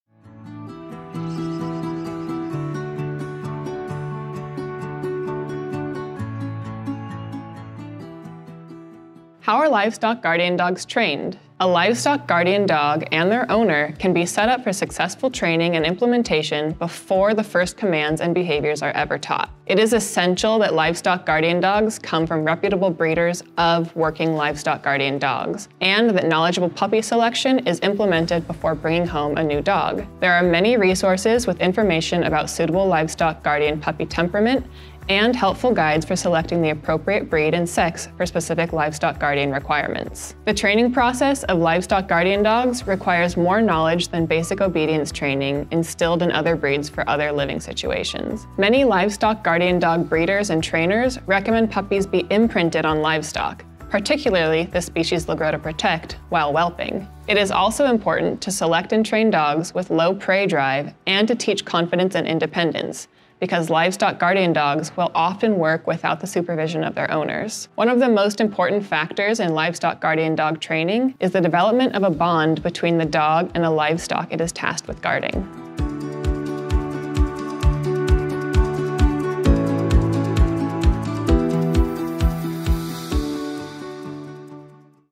How are livestock guardian dogs trained?
Curious about how livestock guardian dogs are trained? 🐶 Join us in exploring this fascinating process with insights from the Cougar Conservancy's Kelly Hood in our Cougar Coexistence Video Series! 🎥
VIDEO SERIES BY: National Wildlife Federation, Cougar Conservancy DIRECTED BY: Korinna Domingo WRITTEN BY: Korinna Domingo, Kelly Hood, Jeff Sikich, Seth Riley, Mark Lotz, Tiffany Yap, Miguel Ordeñana, Johanna Turner, Beth Pratt VIDEO PRODUCTION: Create TheLAB  PRODUCTION ASSISTANT: Kelsey Bernard POST-PRODUCTION: Oliver Sandino, Korinna Domingo PROJECT FUNDED BY: National Wildlife Federation FILMED: September 2022. Thanks to the Santa Monica Mountains National Recreation Area and California Department of Fish & Wildlife for their support and contributions.